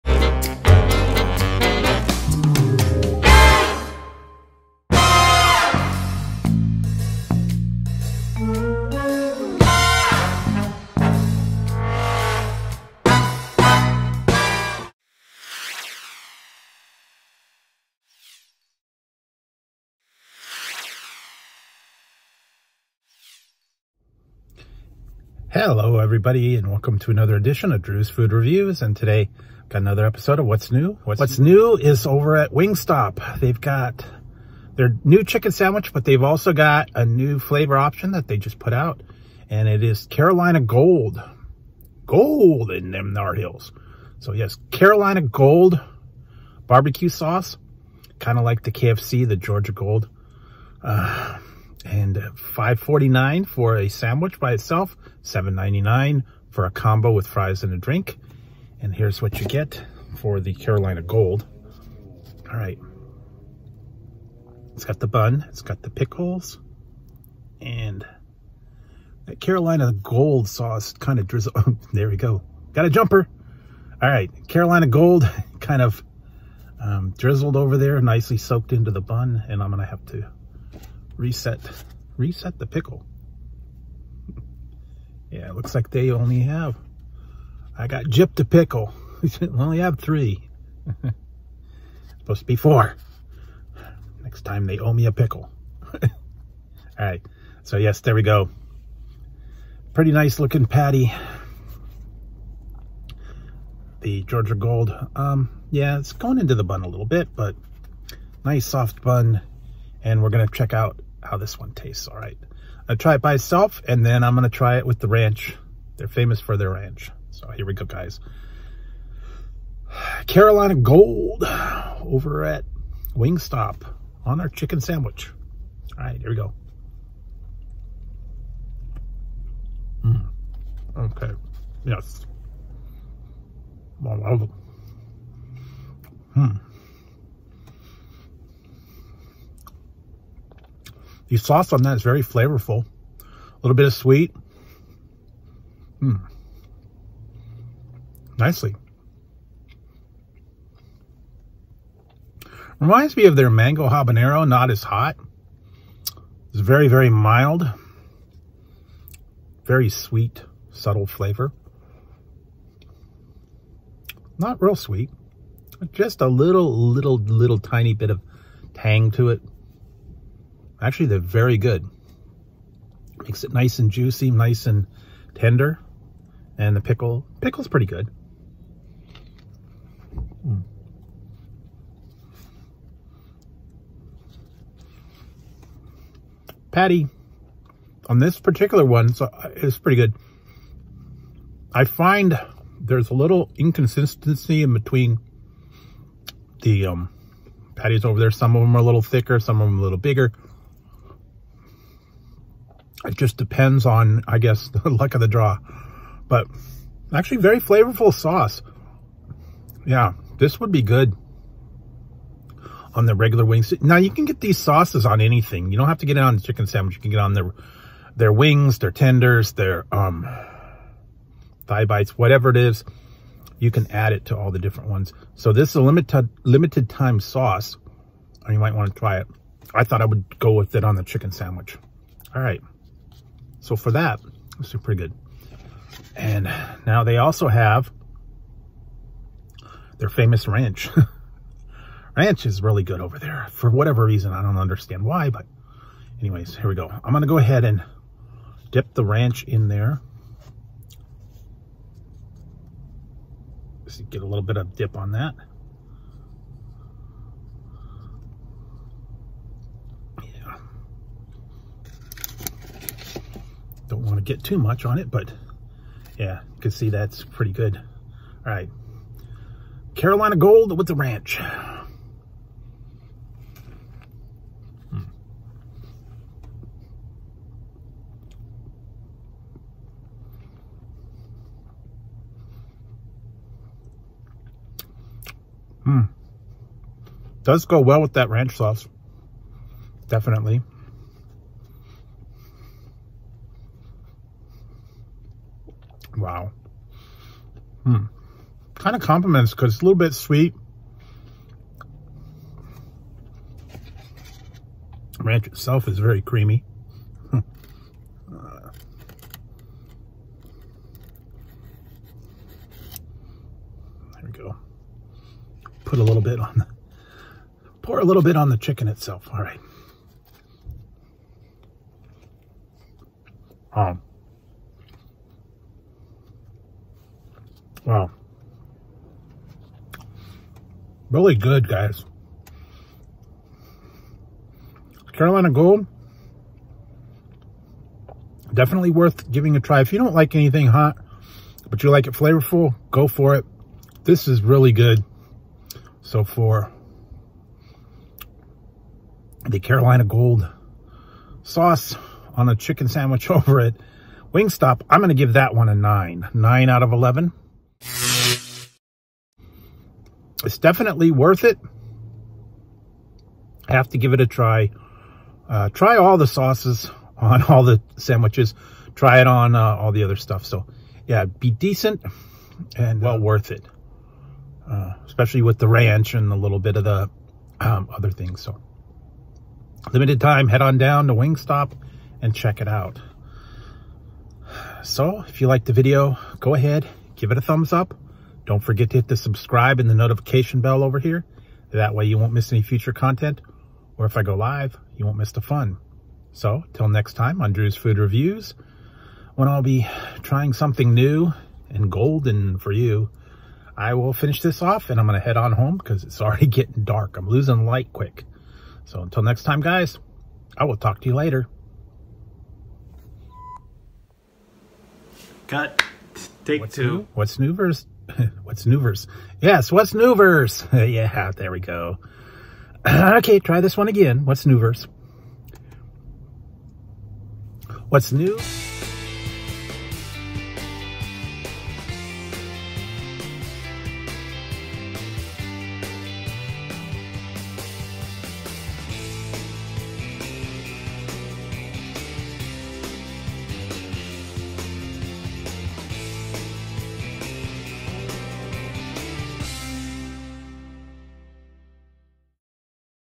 What’s New : Wing Stop Carolina Gold Chicken Sandwich
Today on Whats New I’m at wing stop trying the new Carolina Gold chicken sandwich. Let’s see how I compares to the other ones .